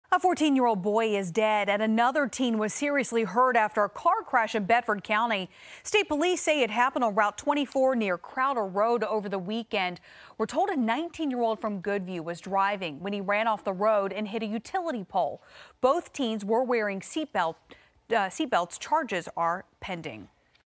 14-year-old killed, 19-year-old injured in Bedford County crash
A teenager is dead and another is in the hospital with serious injuries after a car crash in Bedford County Friday night.